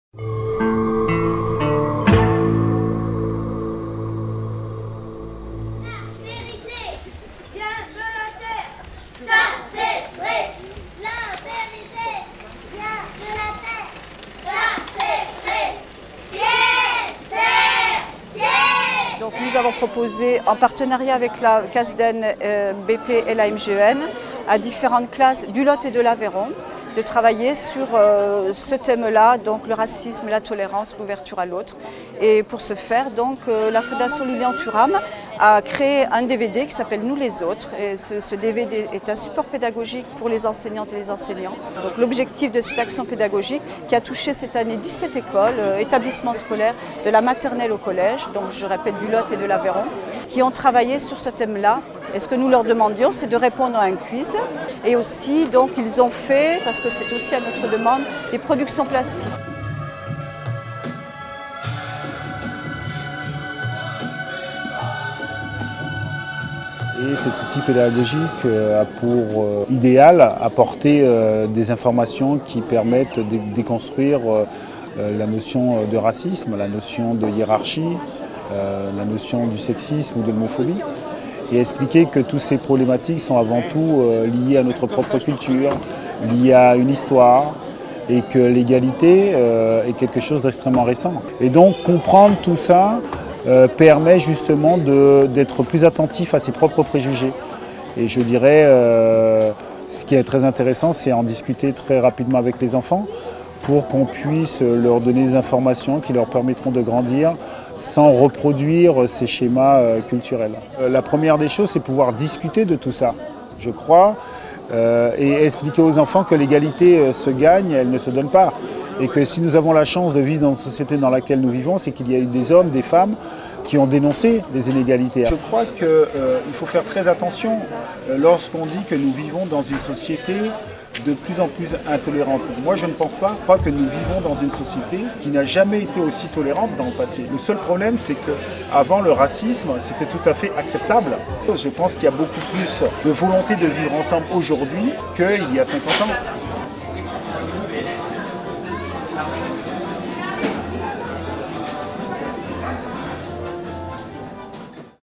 Déconstruire le racisme : Lilian Thuram en visite à Cajarc
La CASDEN et la MGEN ont proposé aux classes de la Maternelle à la 5ème du Lot et d'une partie de l'Aveyron (Villefranchois et Decazevillois) une action « Déconstruire le Racisme » basée le DVD « Nous Autres, Education contre le racisme » réalisé avec la Fondation Lilian Thuram. Une opération menée en collaboration avec le festival Africajarc. La remise des prix a eu lieu le 12 juin 2013 à Cajarc en présence de Lilian Thuram. Reportage de Media 46, une Web-Tv associative rattachée à la Ligue de l'Enseignement de Cahors.